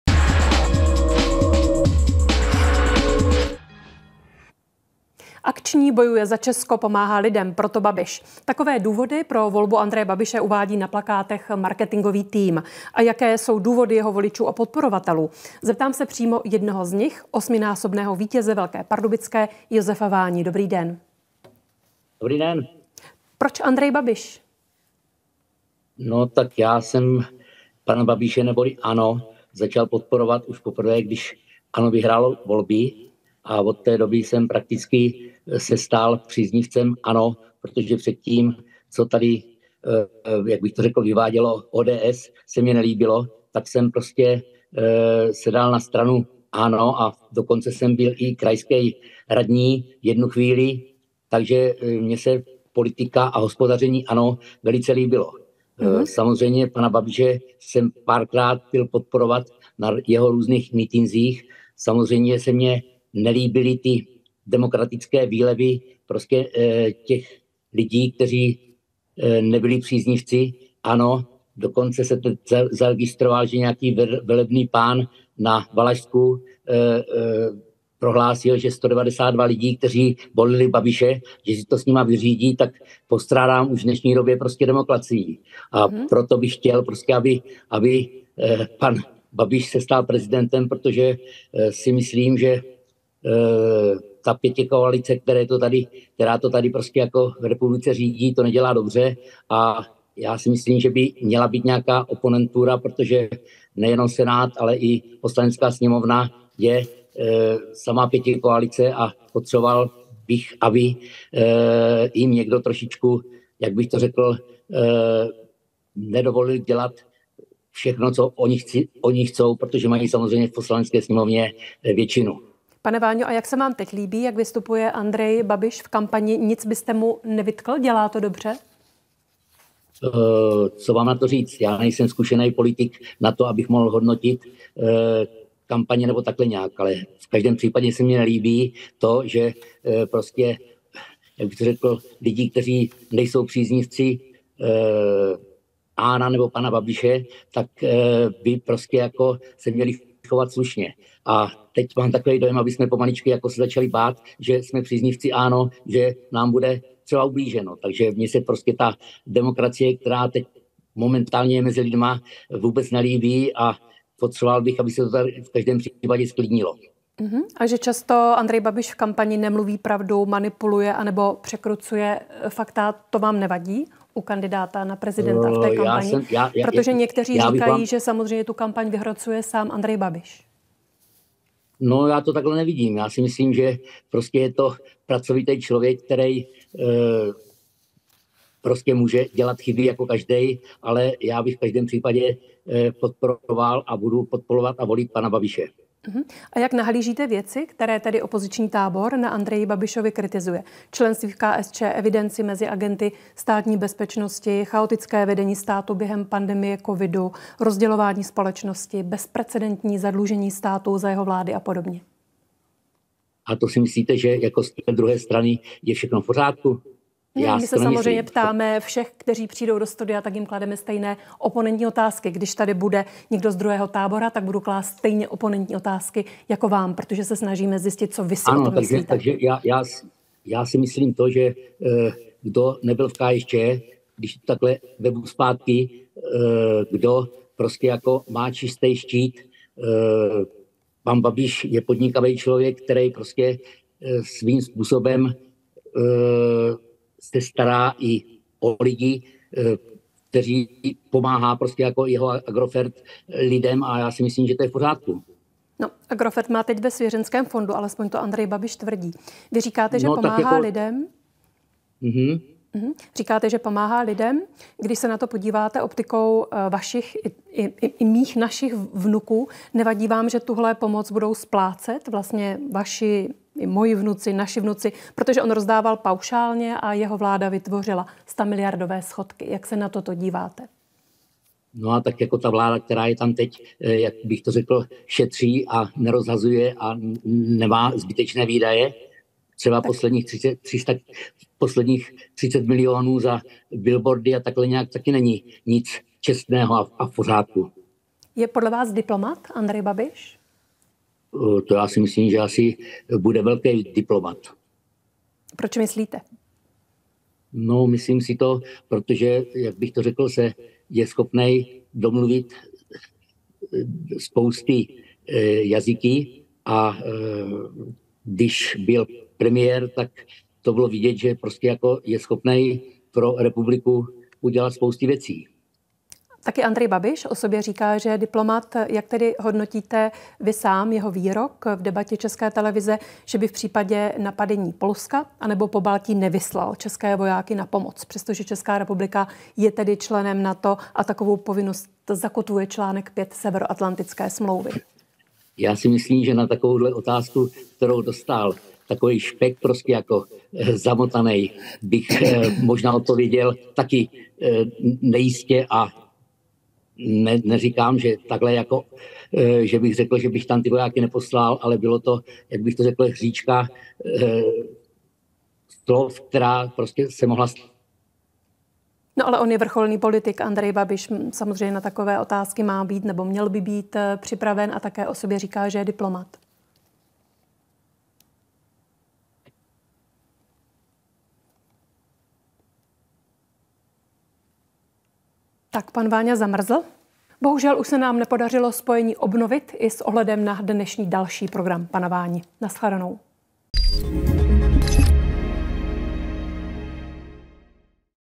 Váňa: Babiš je diplomat, jeho Agrofert pomáhá lidem. Je třeba, aby vládě nedovolil dělat si, co chce
Podpořte novou éru DVTV na Děkujeme!

„Pana Babiše podporuji už od doby, co ANO vyhrálo volby. Jeho politika a hospodaření se mi líbí. Je to velký diplomat, stará se o lidi, jeho Agrofert jim pomáhá. Chci aby se stal prezidentem. Vláda to neřídí dobře a je potřeba, aby jim někdo nedovolil dělat si všechno, co chtějí,” říká žokej Josef Váňa a dodává: „ Nelíbí se mi výlevy lidí, kteří nejsou jeho příznivci. Abychom se skoro začali bát, že nám bude ublíženo. Postrádám v dnešní době demokracii.”